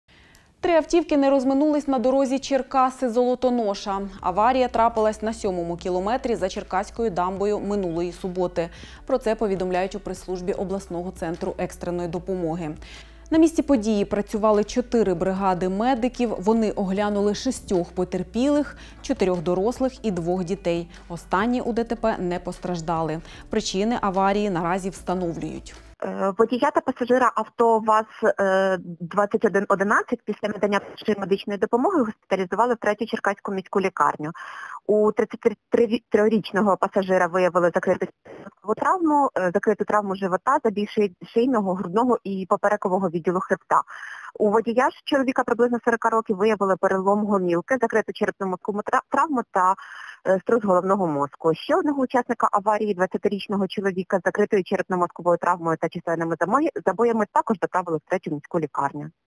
Три автівки не розминулись на автодорозі Черкаси – Золотоноша
Аварія трапилась на 7-му кілометрі за черкаською дамбою минулої суботи